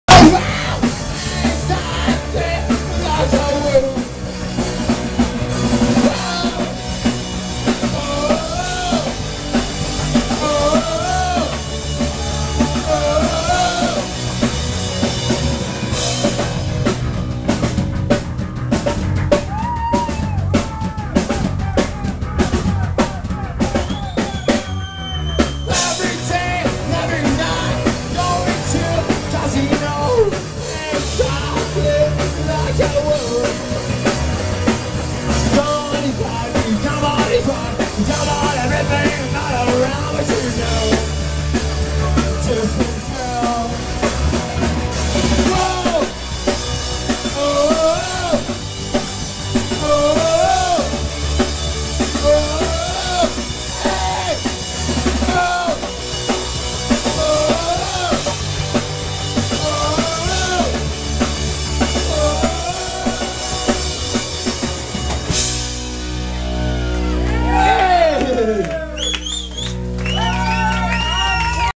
Here are The Deaf Prechers
Voici une vidéo du groupe The Deaf Preachers au Turku Bar à Paris.